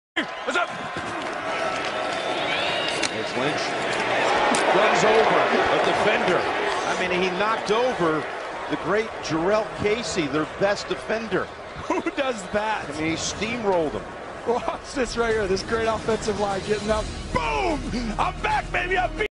Marshawn Lynch RUNS Over Defender Jurrell Casey! BEAST MODE IS BACK!!! Raiders v Titans
Footage courtesy of NFL.